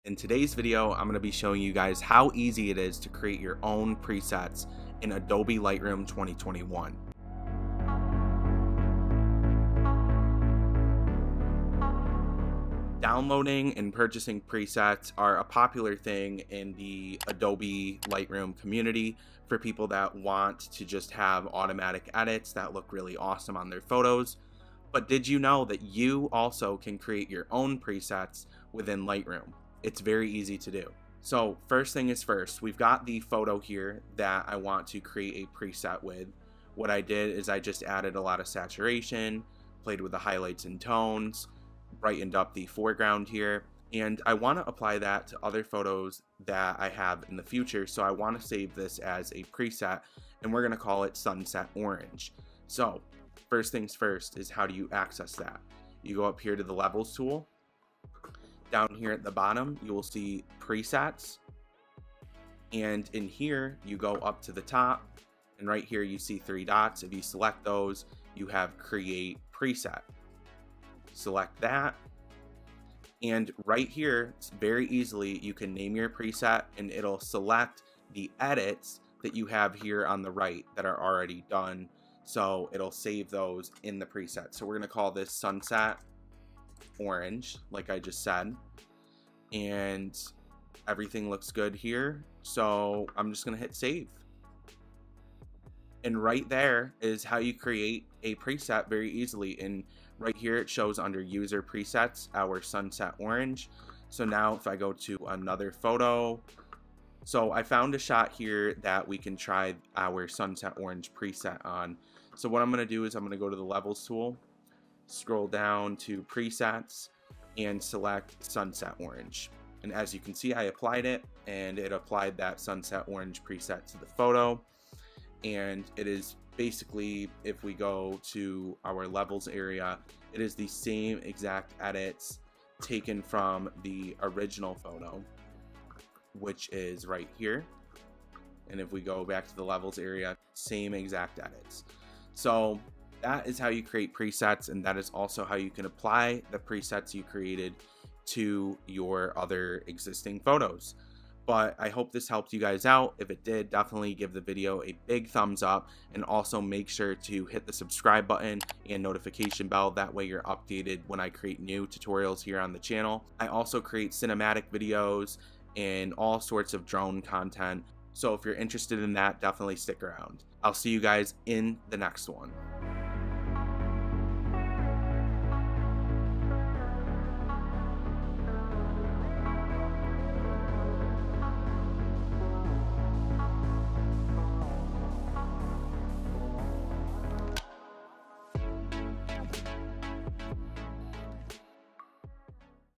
Adobe Lightroom 2021 | Creating Presets
Learn how to create your OWN presets in Adobe Lightroom easily within minutes! You will be a pro at this by the end of the video so make sure to watch it all the way through!

If you are new to the channel my name is Anthony. It helps my video rank higher in the algorithm.